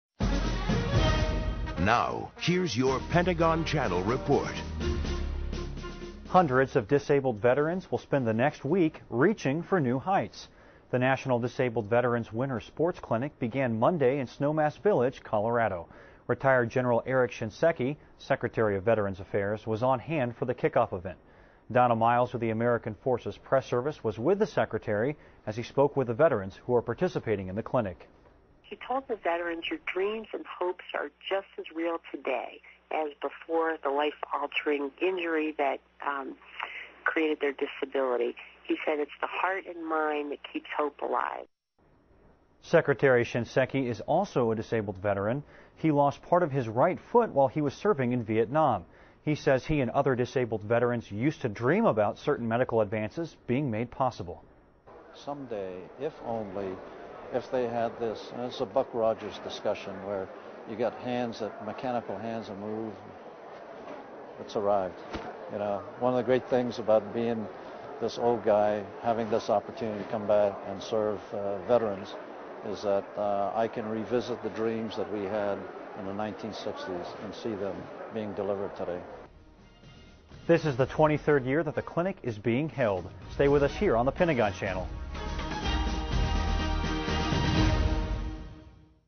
Disabled Veterans Winter Sports Clinic
Hundreds of disabled veterans will spend the next week at the National Disabled Veterans Winter Sports Clinic in Snowmass Village, Colorado.